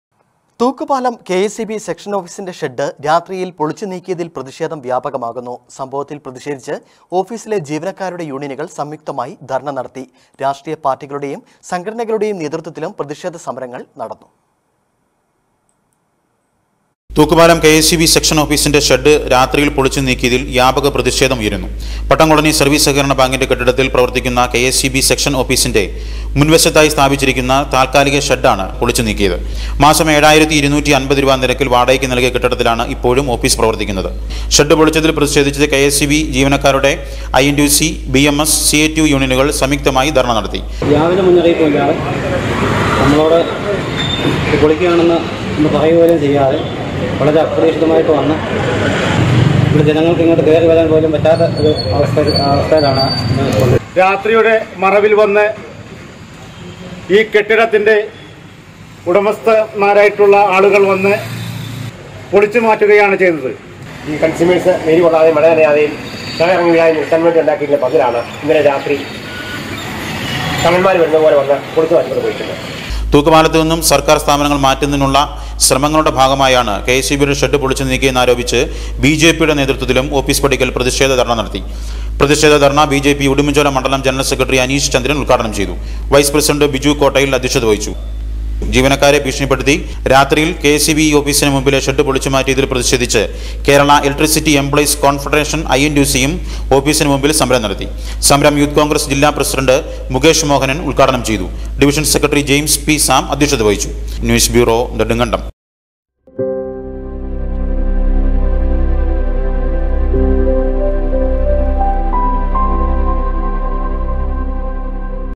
തൂക്കുപാലംKSEB സെക്ഷന്‍ ഓഫീസിന്റെ ഷെഡ് രാത്രിയില്‍ പൊളിച്ചുനീക്കിയതില്‍ പ്രതിഷേധം വ്യാപകമാകുന്നു
സംഭവത്തില്‍ പ്രതിഷേധിച്ച് ഓഫീസിലെ ജീവനക്കാരുടെ യൂണിയനുകള്‍ സംയുക്തമായി ധര്‍ണ നടത്തി..